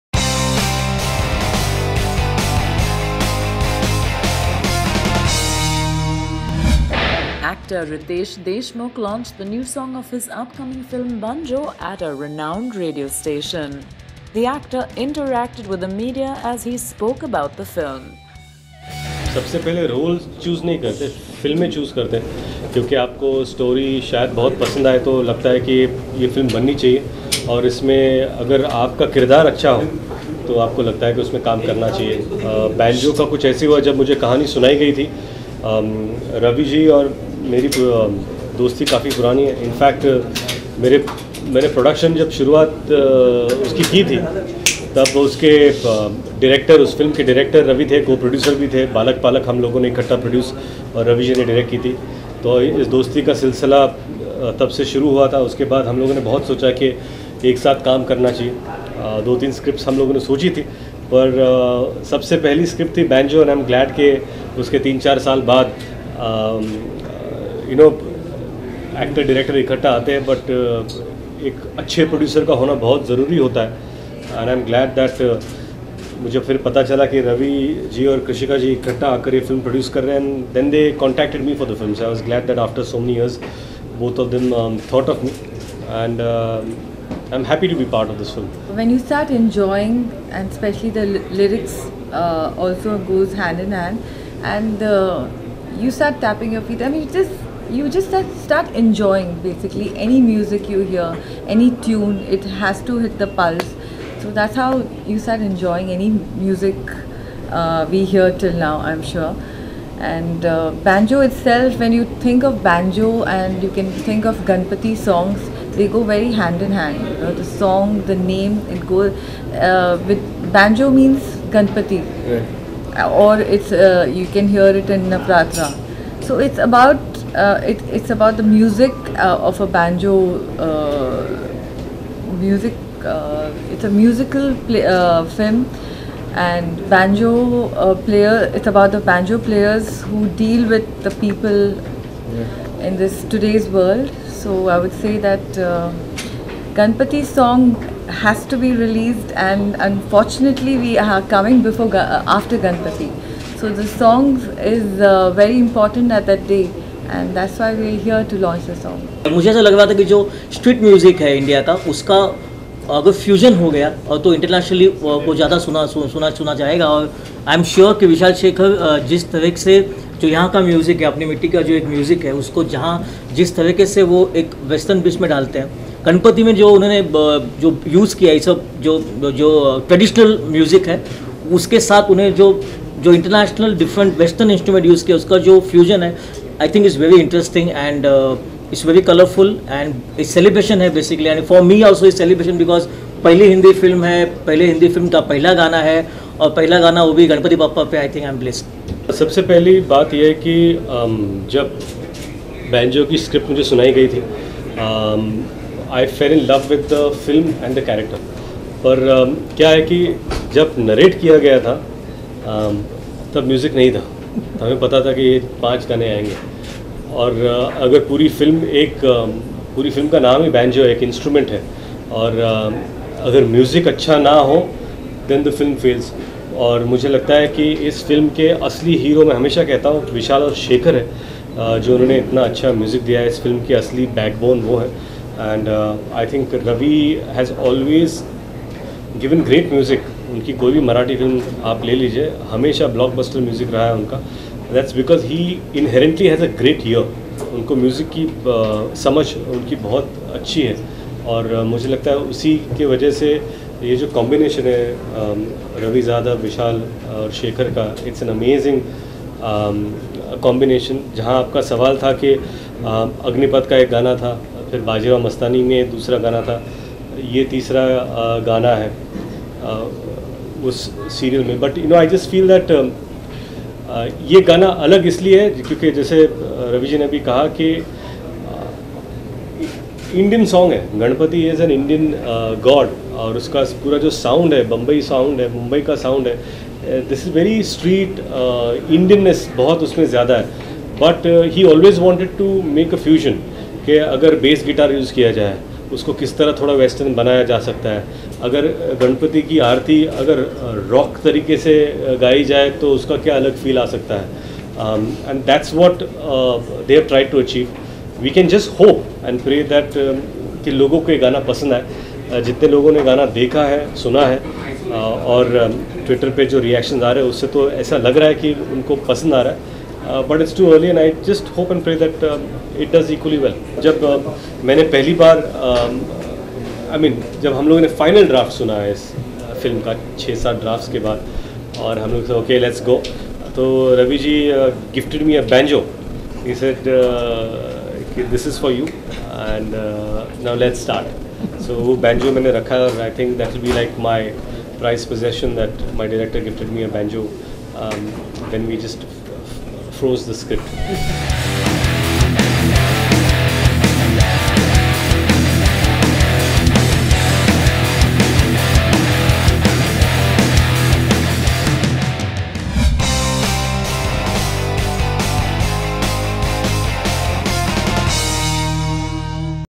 Banjo Song Launch
Meet Riteish Deshmukh at Radio Mirchi as he launches a song from his upcoming film, Banjo.